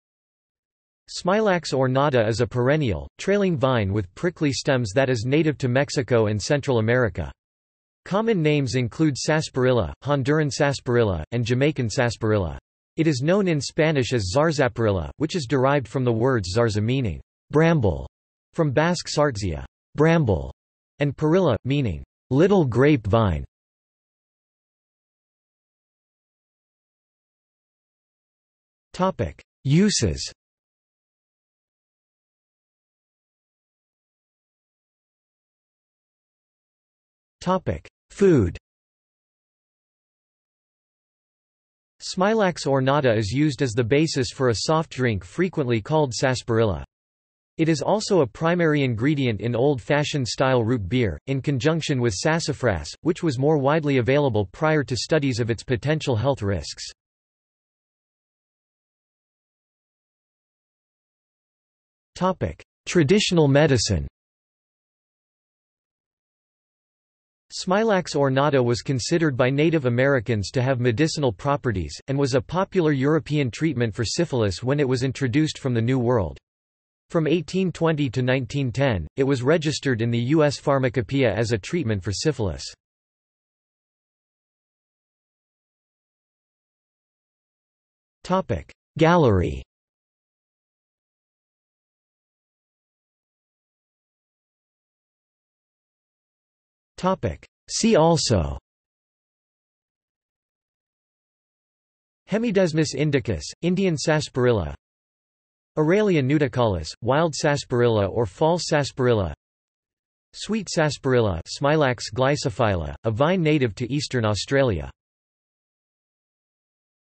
Smilax ornata | Wikipedia audio article
This is an audio version of the Wikipedia Article:
Smilax ornata

00:00:25 1 Uses
00:00:34 1.1 Food
00:00:58 1.2 Traditional medicine
00:01:24 2 Gallery
00:01:33 3 See also

- Socrates

SUMMARY
Smilax ornata is a perennial, trailing vine with prickly stems that is native to Mexico and Central America. Common names include sarsaparilla, Honduran sarsaparilla, and Jamaican sarsaparilla. It is known in Spanish as zarzaparrilla, which is derived from the words zarza meaning "bramble" (from Basque sartzia "bramble"), and parrilla, meaning "little grape vine".